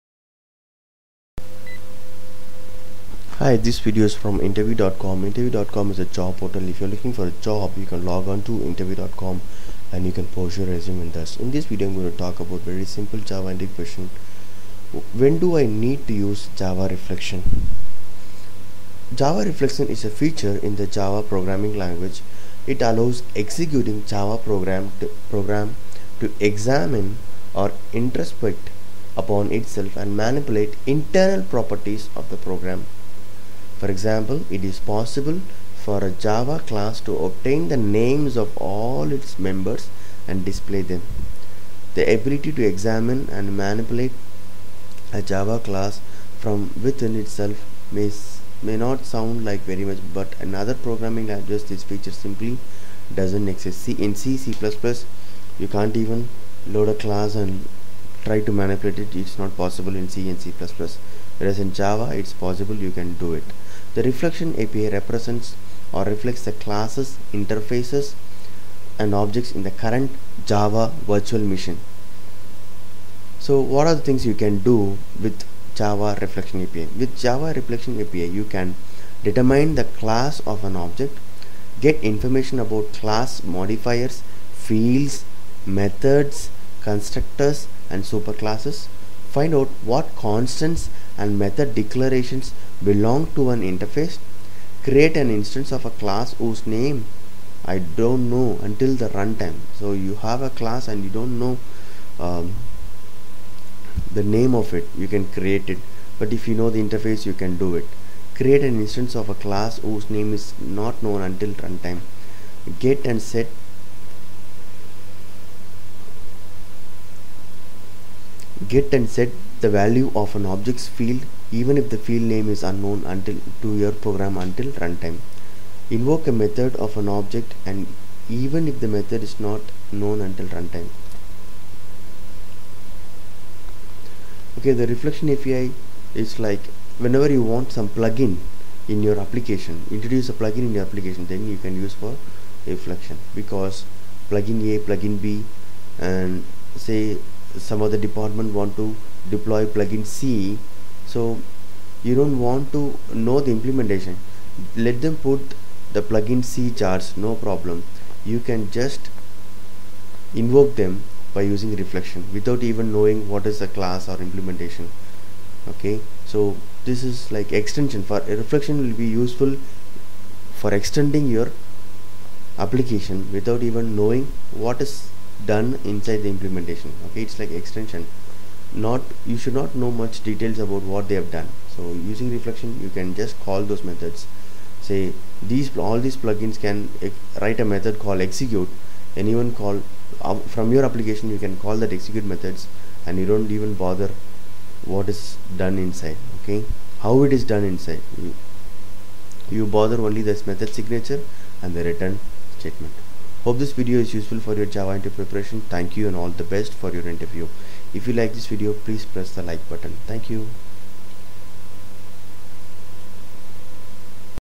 Java Interview Questions And Answers When to use Java Reflection API in your application
to get notifications. Java Interview Questions And Answers When to use Java Reflection API in your application

Reflection is a feature in the Java Programming language.It allows  executing Java program to examine or "introspect" upon itself,and manipulate internal properties of the program.For example it is possible for a Java class to obtain the names of all its members and display them.

The ability to examine and manipulate a Java class from within itself may not sound like very much ,but in other programming languages this feature simply doesn't exist.C,C++,etc

The reflection API represents,or reflects, the classes,interface, and objects in the current Java Virtual Machine.

With reflection API you can :

Determine the class of an object.

Get information about a class's modifiers,fields,methods,constructors,and superclasses.

Find out  what constants and method declarations belong to an interface.

Create an instance of a class whose name is not known untill runtime.

Get and set the value of an object's field,even if the field name is unknown to your program untill runtime.

Invoke a method on an object,even if the method is not known untill runtime.